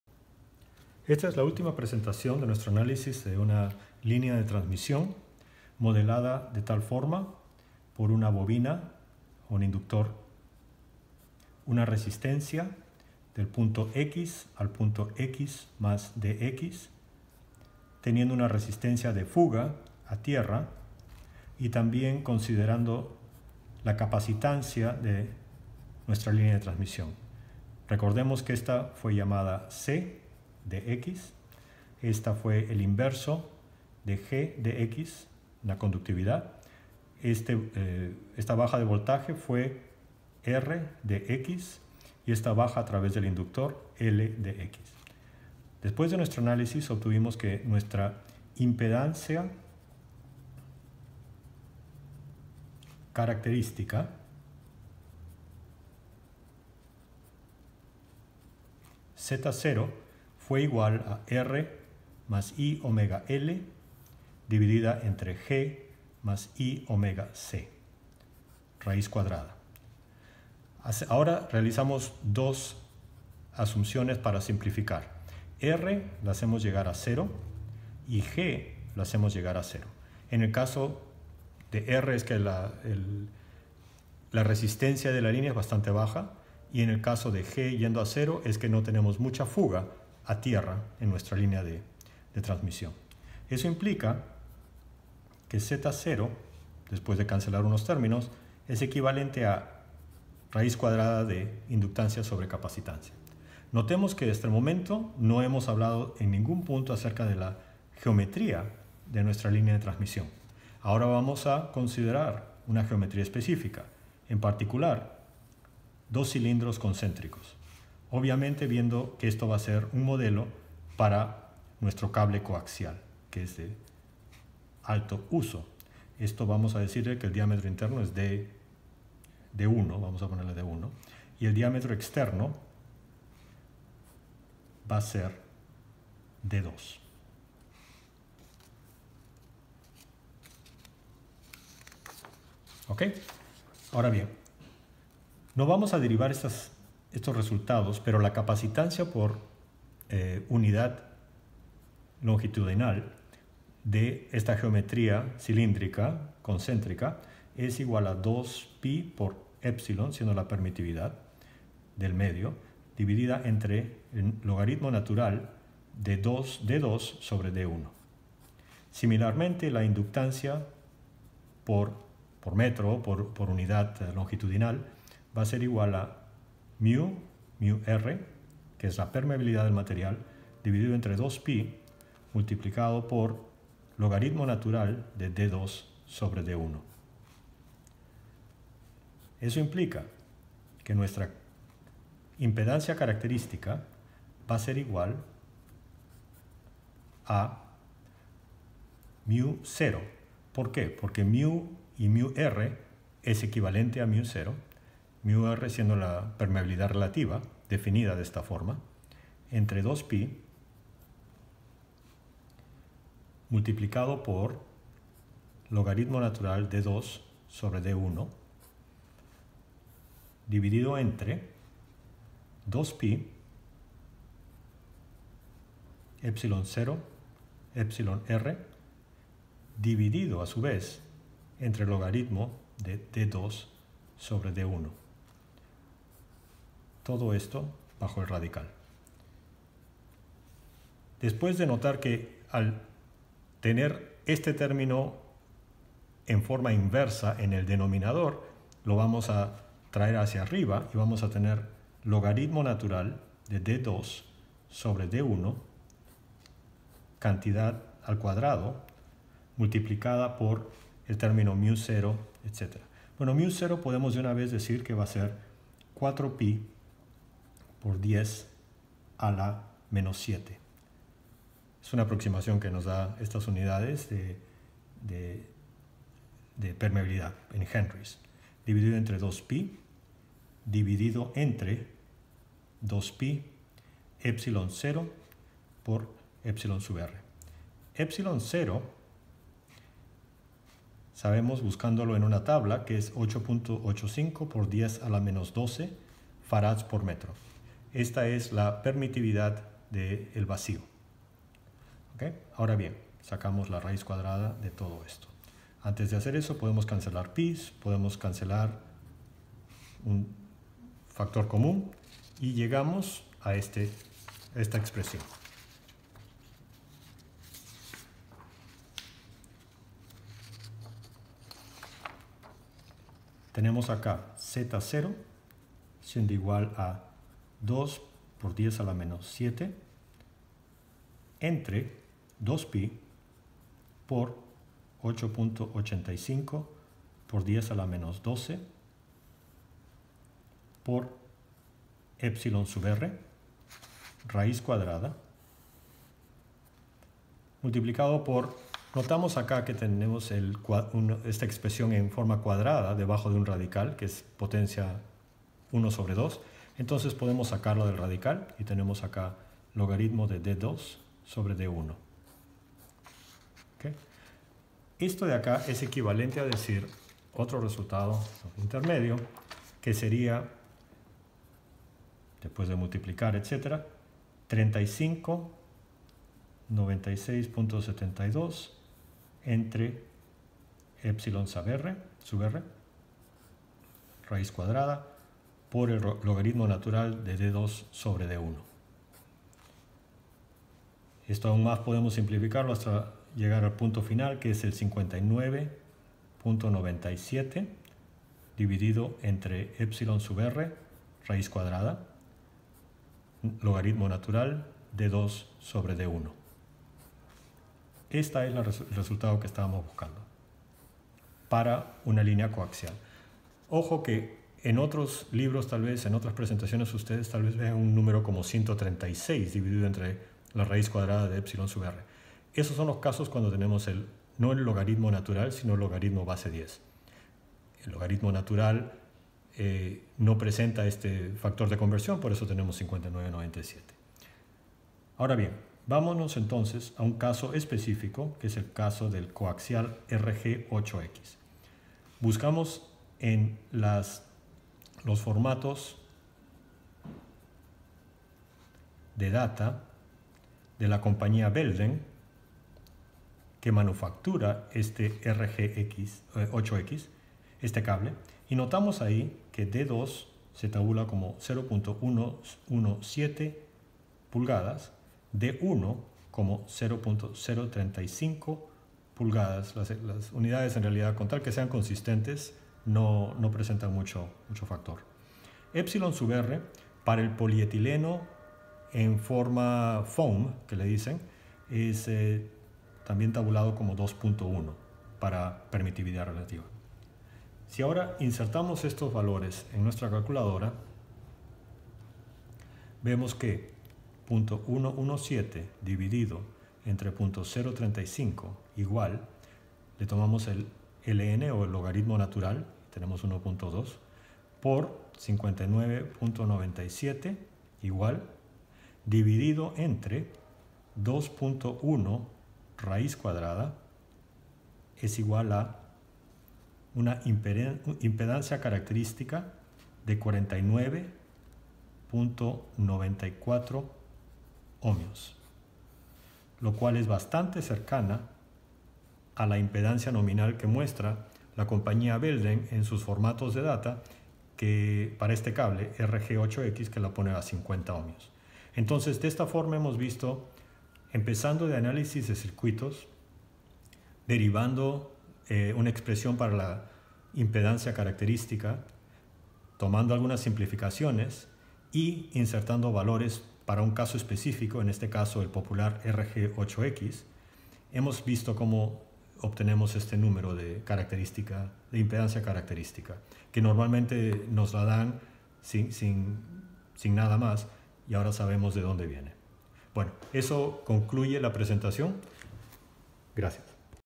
Zo del cable coaxial RG-8X - Parte 3
Este video muestra como obtener la impedancia caracteristica de "50 ohmios" para un cable RG-8X de la compañía Belden.  Empieza con la ecuación de la impedancia característica derivada en videos previos y finaliza con el valor de 50 ohms.